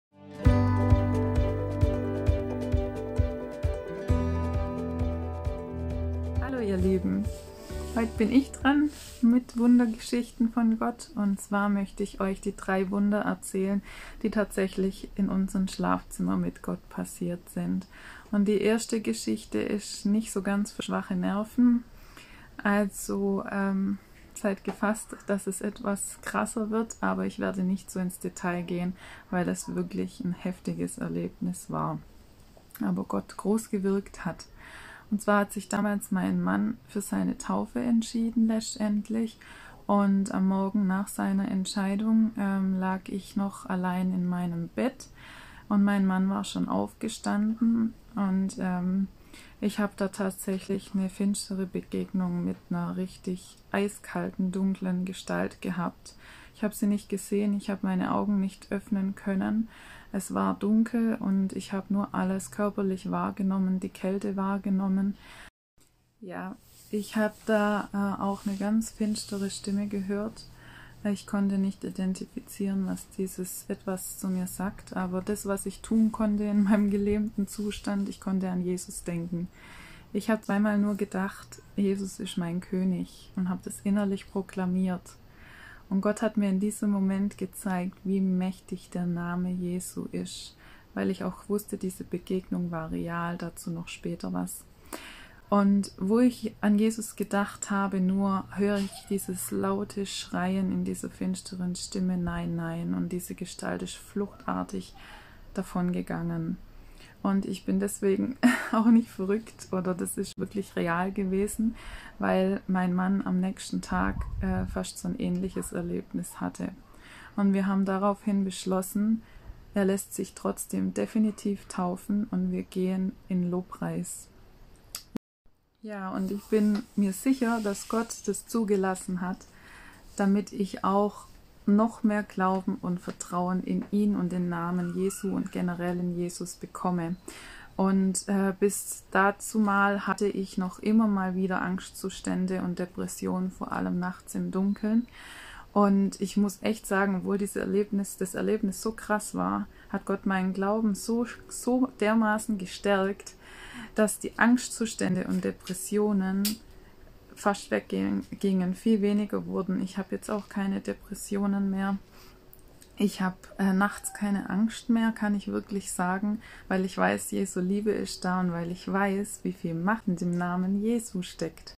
Es ist Kraft im Namen jesus
Eine dunkle Gestalt legte sich zu mir und ich konnte meine Augen und meinen Mund nicht öffnen. Das einzige woran ich denken konnte war: Jesus ist mein König! Nachdem ich seinen Namen zweimal in Gedanken gerufen habe, ließ die Dunkelheit von mir ab und seither bin ich frei von Depressionen und meiner Angst im Dunkeln. Alle Ehre sei Gott.

Dieses Video ist Teil der "Themenwoche: Wunder" auf Instagram gewesen. Da es für uns alle sehr ermutigend war und unseren Glauben gestärkt hat wollten wir die Zeugnisse auch hier teilen.
Mehr Infos zur Person findet ihr auf Instagram @linesbylove
Ihre Vision ist es Menschen durch Zeugnisse und Letteringart zu erreichen. Sie liebt die Bibel und die Gemeinschaft mit Jesus.